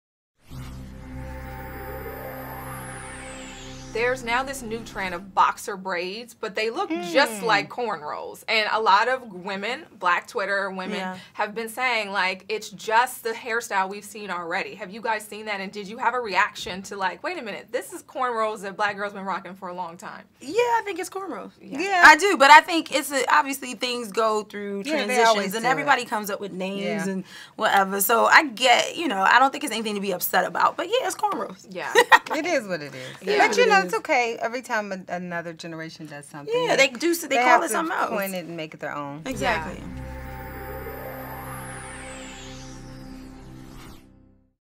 What Regina Hall & Eve Have To Say About Boxer Braids & Cultural Appropriation
HipHollywood Caught up with "Barber Shop: The Next Cut" stars Eve and Regina Hall about cultural appropriation and all the controversy surrounding women wearing and calling corn rows boxer braids